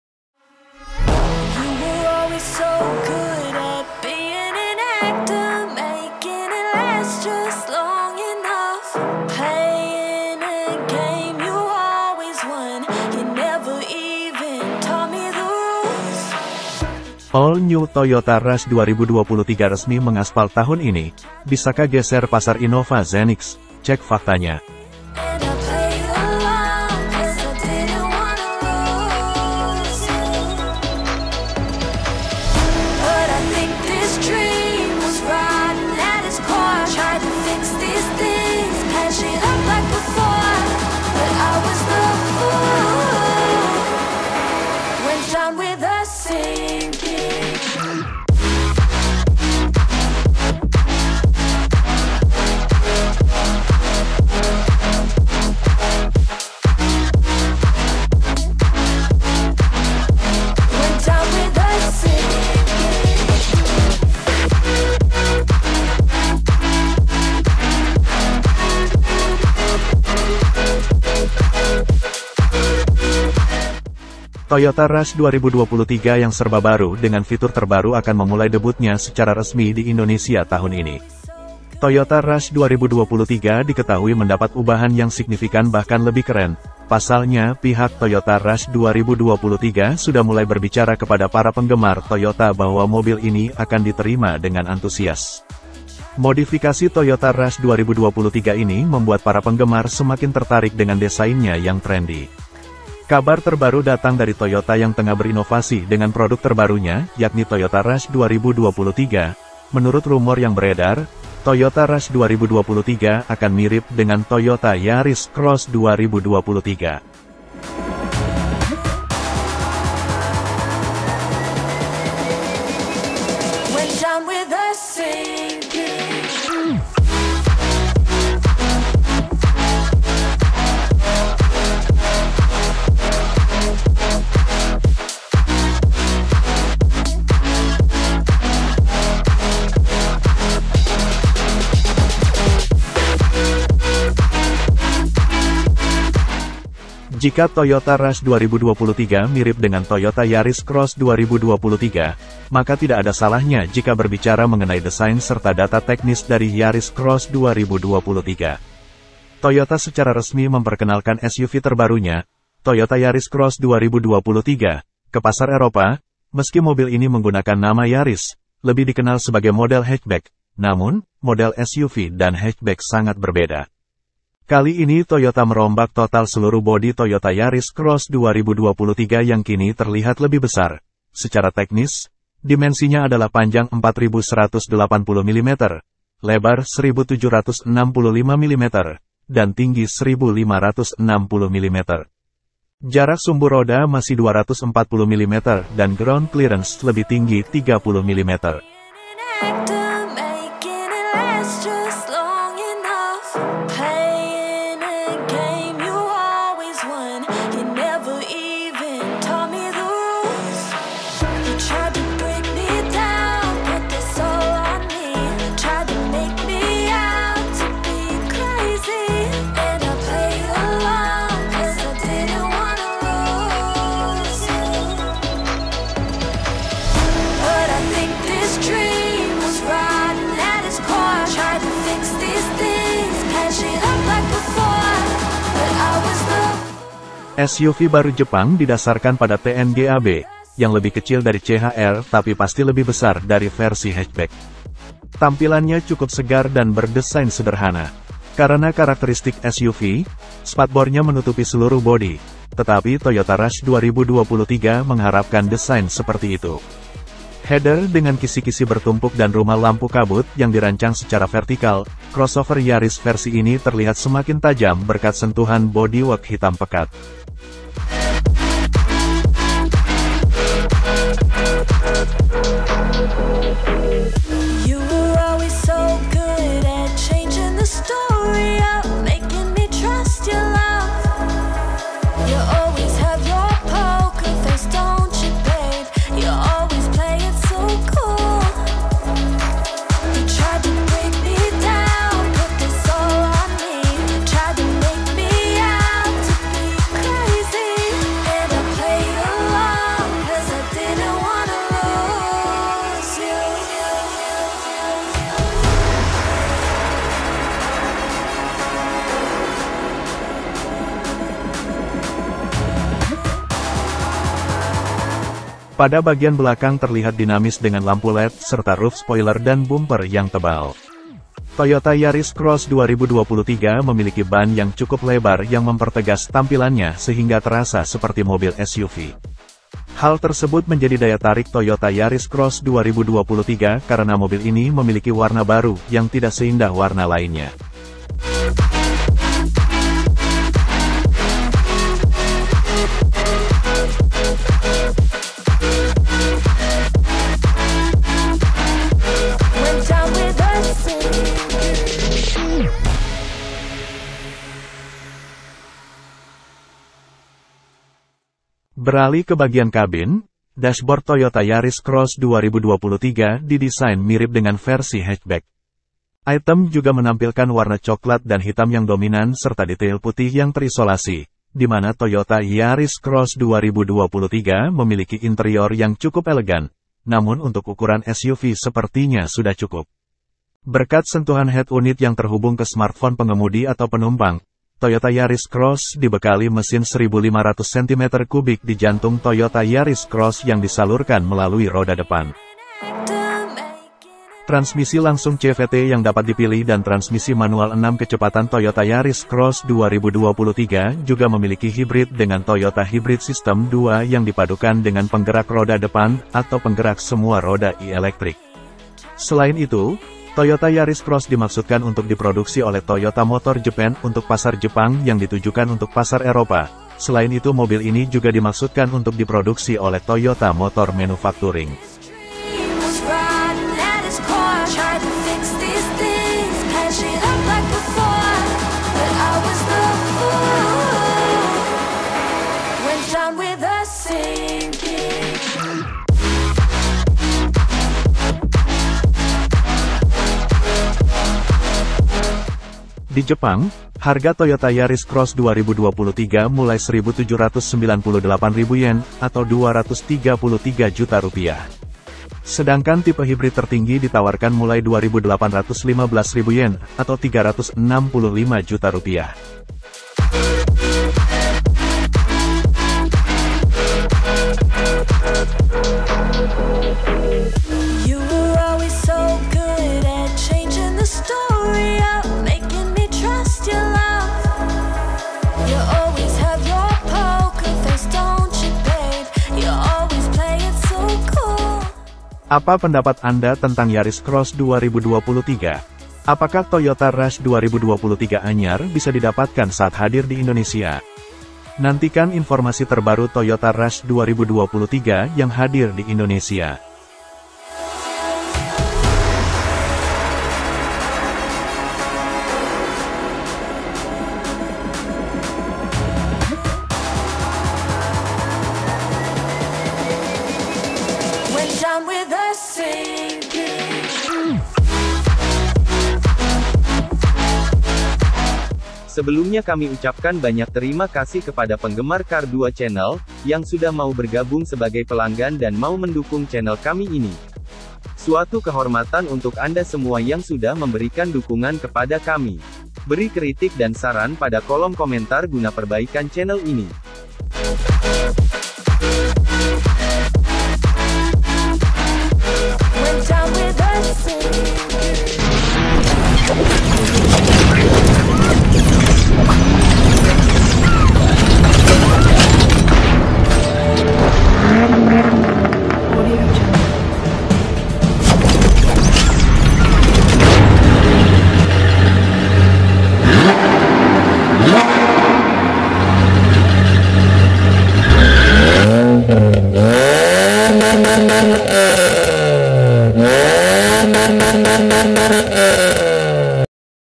All New Toyota Rush 2023 Resmi Mengaspal Tahun ini
Toyota Rush 2023 yang serba baru dengan fitur terbaru akan memulai debutnya secara resmi  di Indonesia tahun ini.
  Toyota Rush 2023 diketahui mendapat ubahan yang  signifikan bahkan lebih keren, Pasalnya pihak Toyota Rush 2023 sudah mulai berbicara kepada para penggemar Toyota bahwa mobil ini akan diterima dengan antusias. Modifikasi Toyota Rush 2023 ini membuat para penggemar  semakin tertarik dengan desainnya yang trendy.

 Kabar terbaru datang dari Toyota yang tengah berinovasi dengan produk terbarunya yakni Toyota Rush 2023, Menurut rumor yang beredar, Toyota Rush 2023 akan mirip dengan Toyota Yaris Cross 2023.